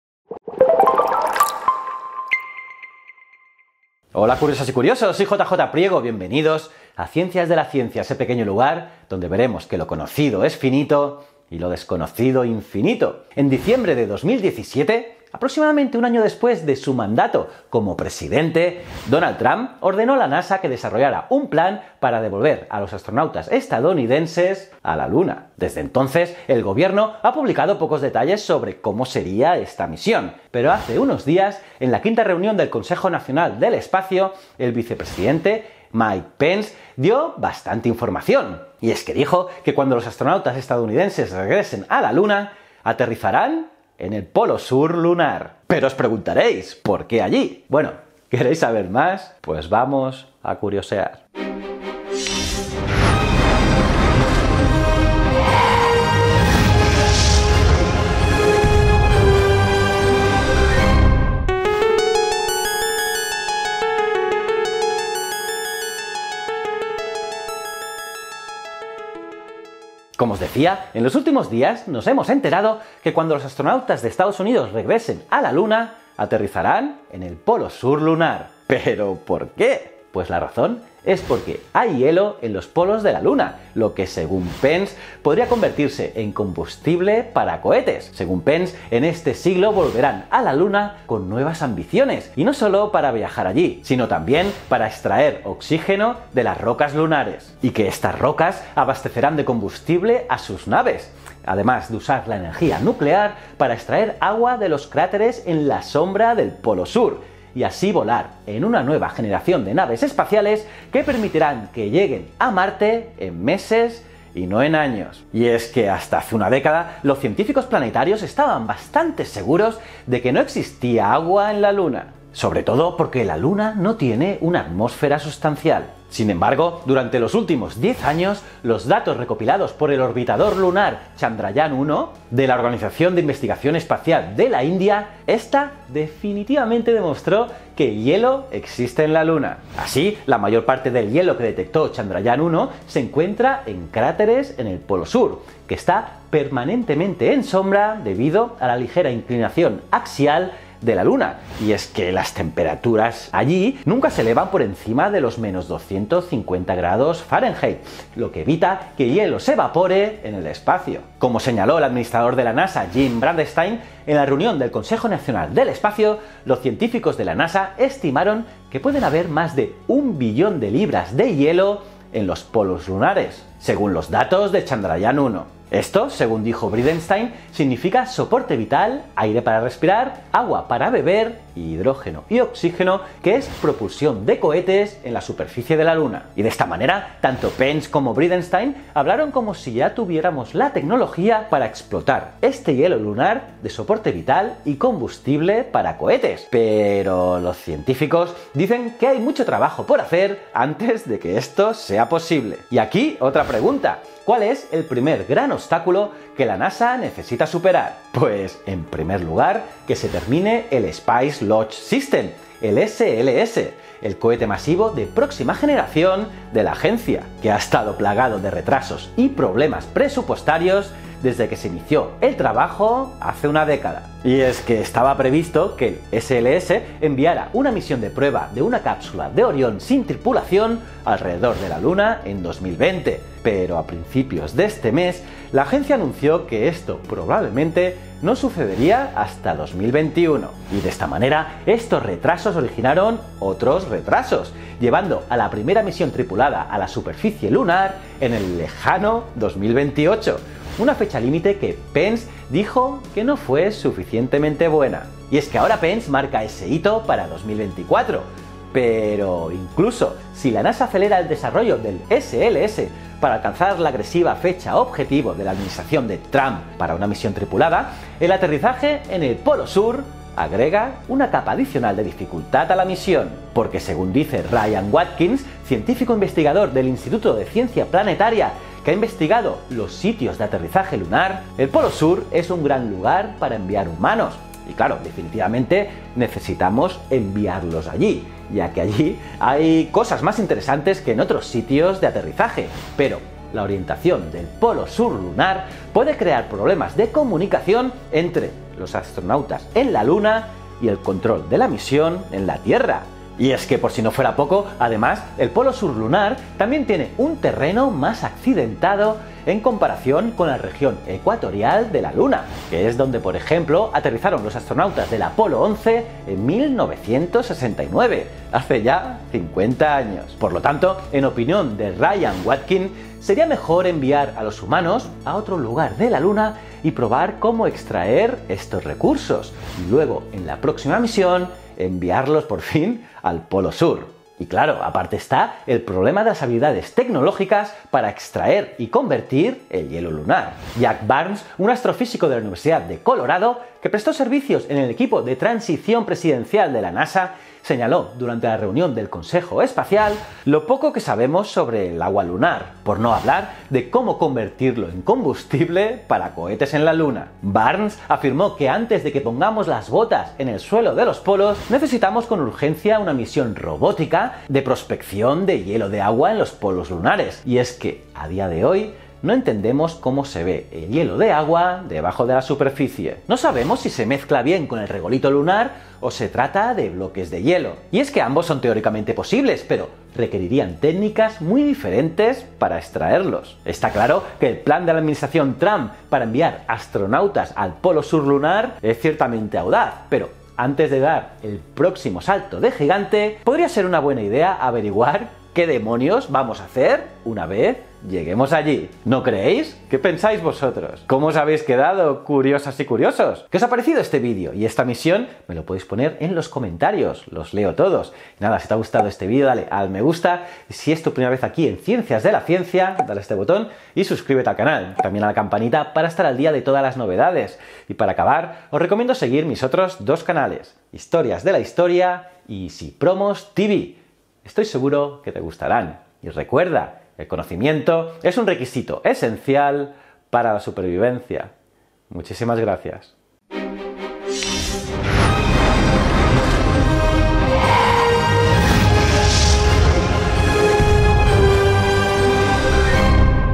¿Por qué IR al POLO SUR de la LUNA? | La nueva misión de la NASA y DONALD TRUMP
¿Por qué la NASA quiere enviar astronautas al polo sur de la Luna? ¿Hay agua? ¿Quieren extraer oxígeno de las rocas lunares? ¿Es real el proyecto?

𝓜𝓾𝓬𝓱𝓪𝓼 𝓰𝓻𝓪𝓬𝓲𝓪𝓼

𝓙𝓙 😘👌